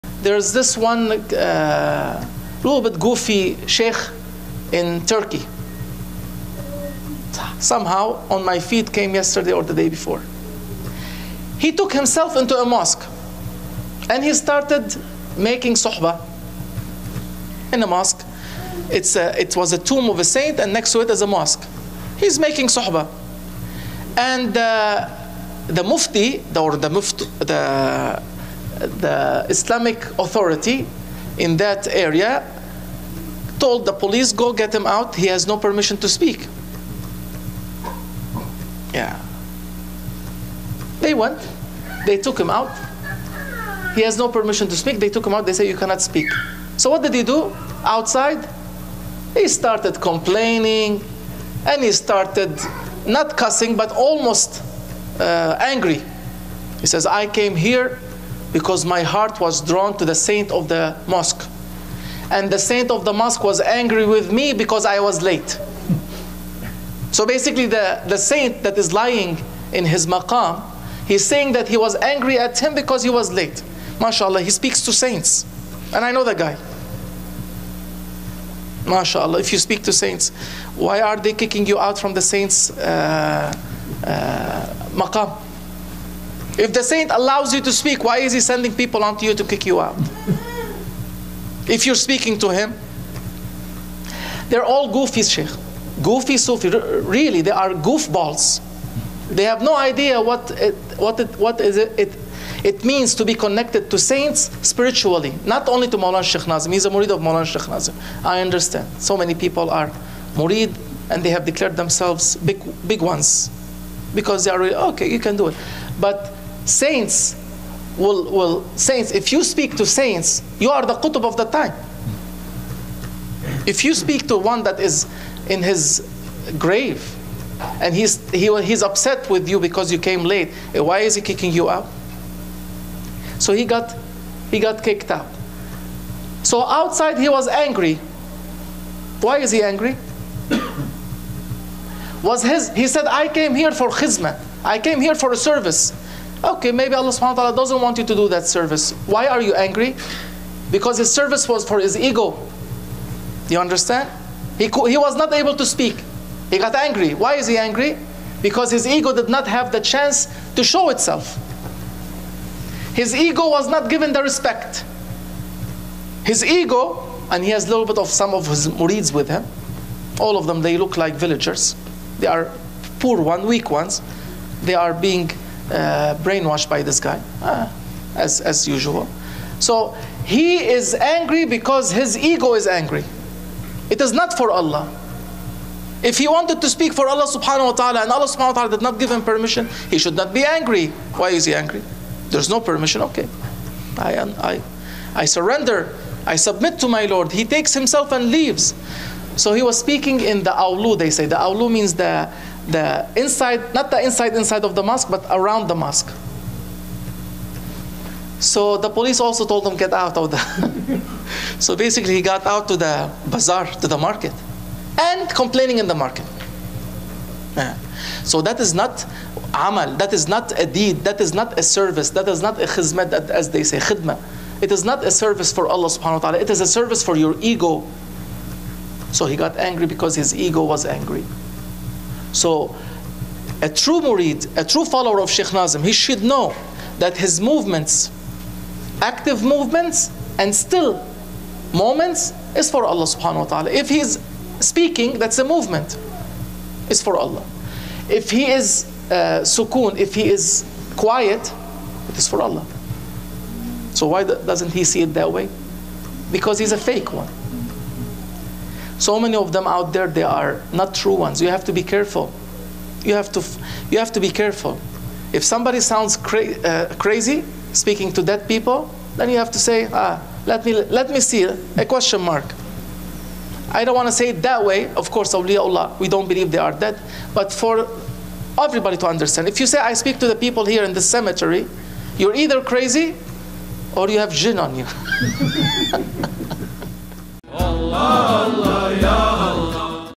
Hati-Hati dengan Goofy Sufi | Banyak Syekh Palsu di Luar Sana
Shaykh Nour Kabbani
Zawiyah Fenton, Michigan